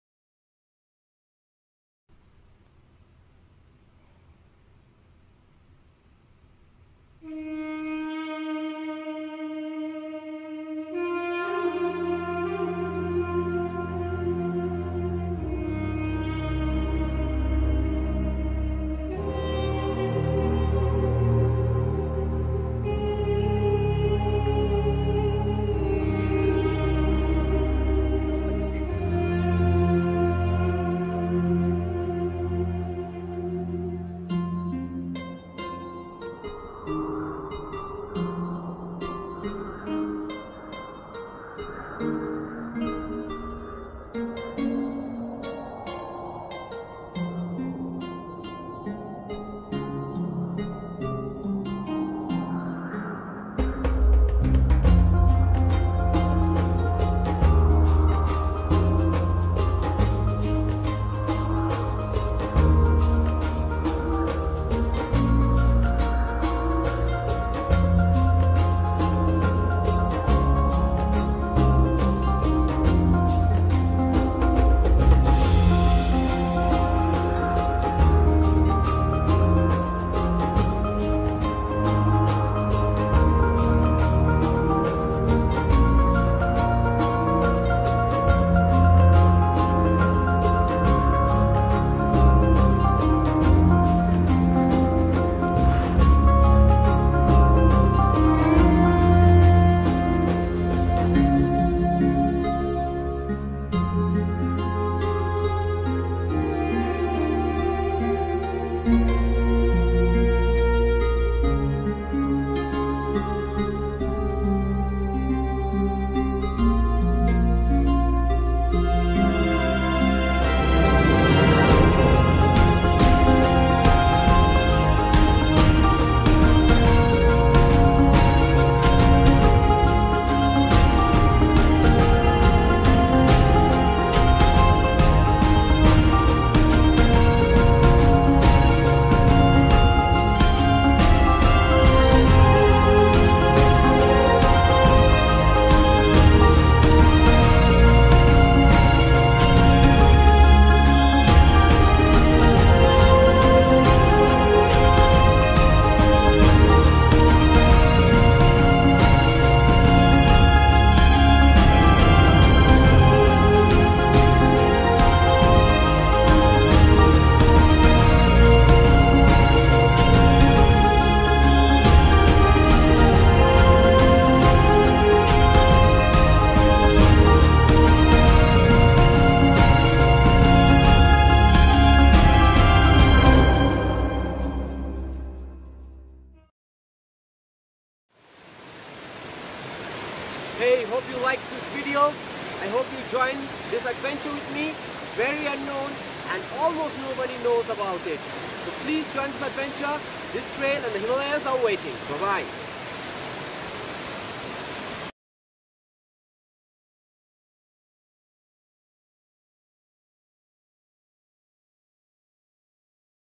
Unknown Alpine Lake of Rorag Sar 14400 feet .
The Unknown alpine lake of Rorag Sar doesn't make it to bucketlist of regular trekkers, but that is exactly why it's beauty is pristine and untouched yet. Those who look to trek to places less known and unventured, Rorag Sar provides a perfect opportunity.

Magnificent views of panaromic high peaks of Parvati valley such as Rudranag , Papsura , Dharamsura and others make a jaw dropping back drop. Pin Parvati glacier , Saraungma Glacier and Malana Glacier is also visible on a clear day.

All on All , this 4 night 5 days trek make up for one of the grandest adventure in Parvati valley.
Consult me to plan for this trek or such kind of high altitude Adventures.